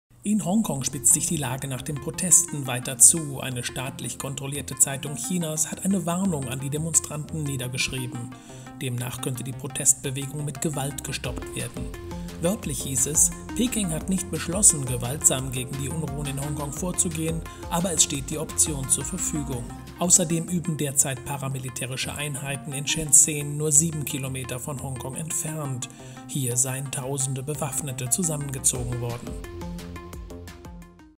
Warnung aus Peking nach Hongkong?
In Hongkong spitzt sich die Lage nach den Protesten weiter zu. Eine staatlich kontrollierte Zeitung Chinas hat eine Warnung an die Demonstranten niedergeschrieben...Demnach könnte die Protestbewegung mit Gewalt gestoppt werden.  Wörtlich hiess es: "Peking hat nicht beschlossen gewaltsam gegen die Unruhen in Hongkong vorzugehen aber es steht die Option zur verfügung..." Ausserdem üben derzeit paramilitärische Einheiten in Shenzen nur 7 km von Hongkong entfernt. Hier seien tausende Bewaffnete Einsatzkräfte zusammengezogen worden.